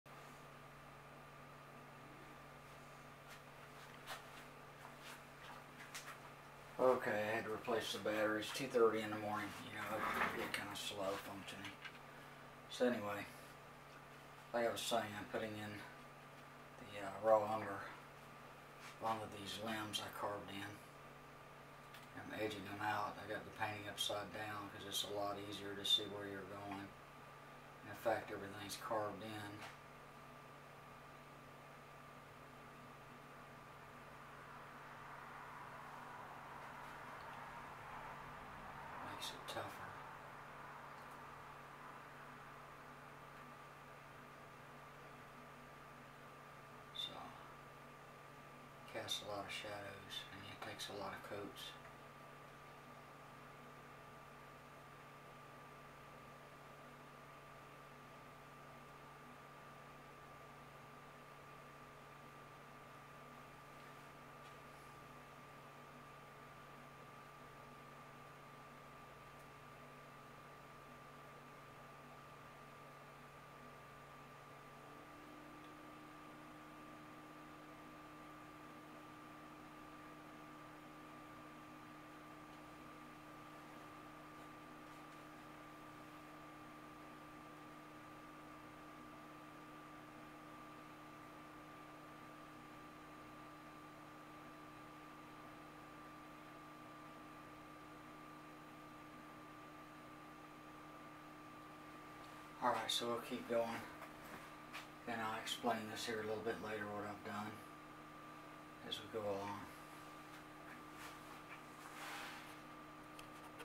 01-17-2017 Painting in the limbs
Up all night painting in the limbs with the first coat on Daniels Speck Painting.